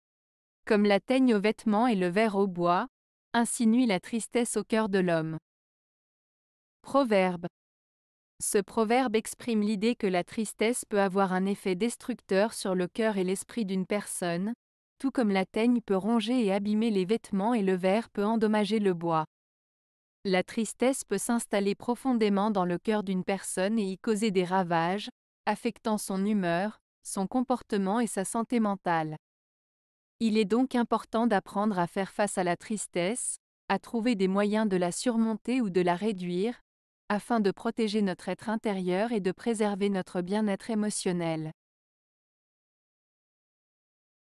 Comme la teigne au vêtement et le ver au bois, ainsi nuit la tristesse au coeur de l'homme.
Ce proverbe exprime l'idée que la tristesse peut avoir un effet destructeur sur le cœur et l'esprit d'une personne, tout comme la teigne peut ronger et abîmer les vêtements et le ver peut endommager le bois. La tristesse peut s'installer profondément dans le cœur d'une personne et y causer des ravages, affectant son humeur, son comportement et sa santé mentale. Il est donc important d'apprendre à faire face à la tristesse, à trouver des moyens de la surmonter ou de la réduire, afin de protéger notre être intérieur et de préserver notre bien-être émotionnel.